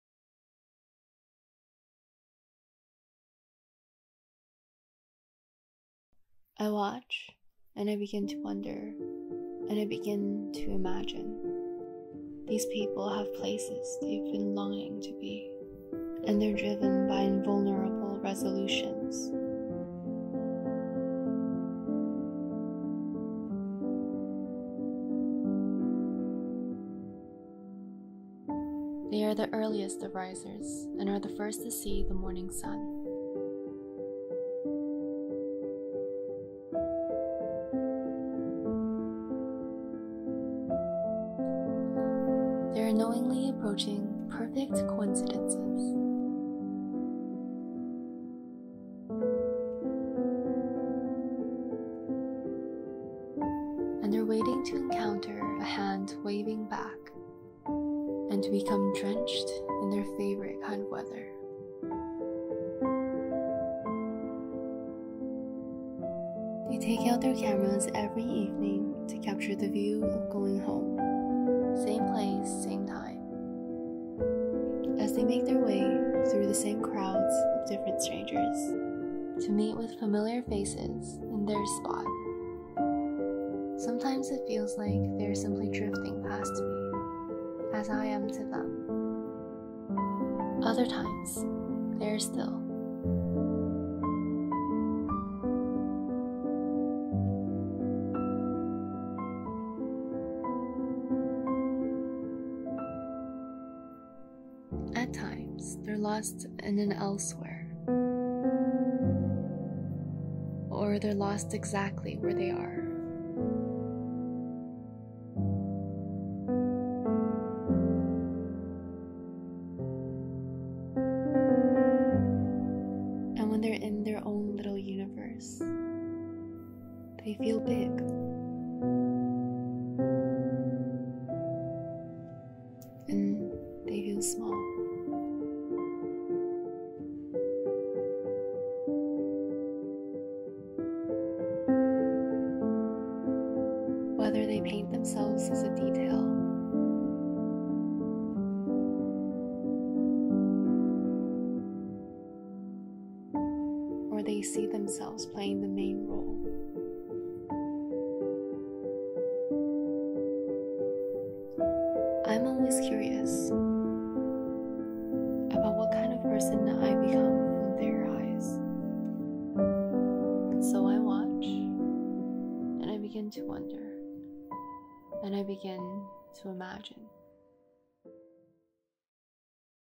PKU 30-Day Challenge | Encounter
How does it feel to perceive your own life as if seen through a stranger's eyes?

"When people are watching, it feels as if I am looking into a world that I am not part of. A gentle, yet unsettling sense of ephemerality dawns, and I instinctively drift towards thoughts of the ordinary." Within every encounter, there lingers a whisper of destiny—a subtle yet profound revelation, concealed in the depths of the everyday.

✨ "Encounter" by Tong Chen, an exchange student at PKU, invites you to step into the shoes of an outsider and witness the world with fresh, uncharted eyes. Don't miss this captivating journey as we dive into the hidden nuances of everyday life and explore the intriguing connections that close the distance between strangers.

WeChat: PKU_Eng